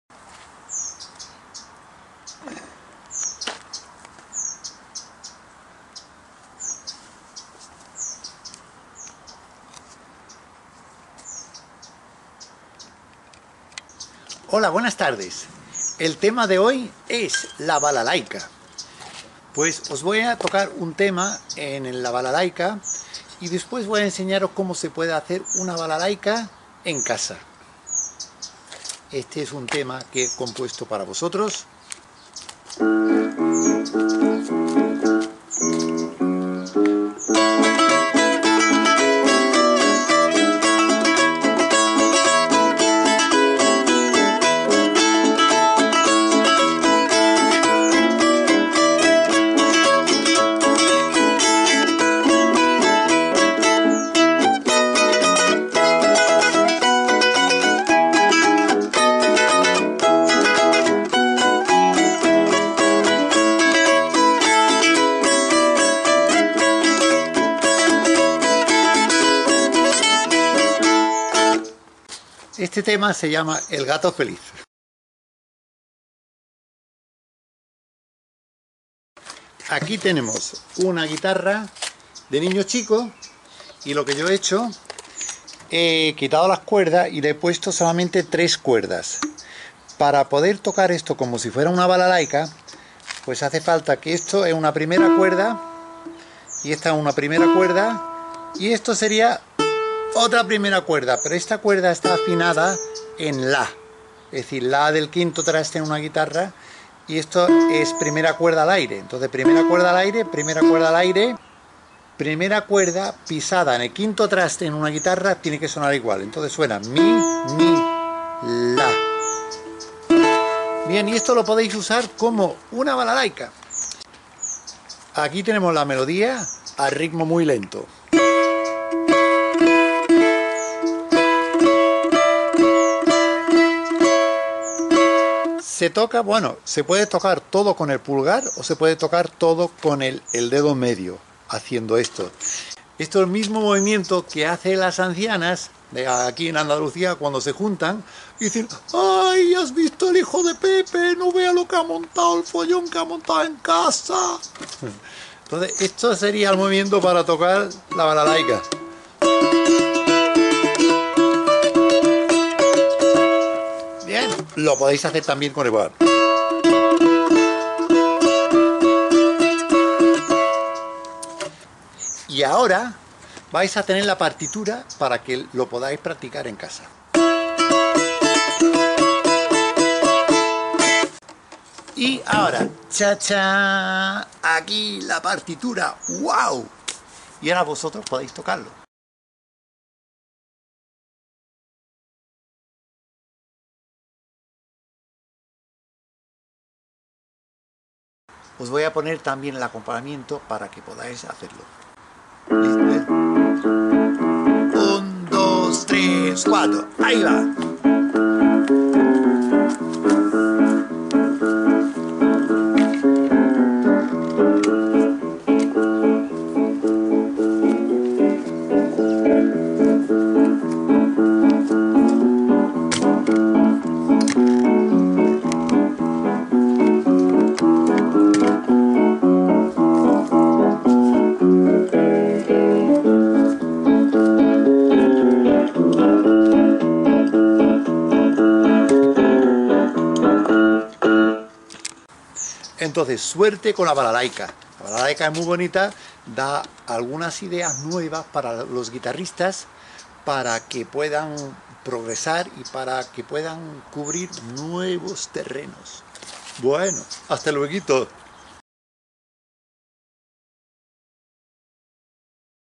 Hazte tu propia balalaika y aprende tocarla
Una guitarra normal (preferiblemente de talla pequeña) se convierte en una balalaika en menos de 5 minutos. El vídeo enseña la técnica de la mano derecha y trae la partitura por TAB con acordes de acompañamiento. También incluye el "jam track".

El vídeo esta destinado a guitarristas y otros cordómanos que quieren explorar las posibilidades de otros instrumentos.

El tema y el contenido es propiedad del autor.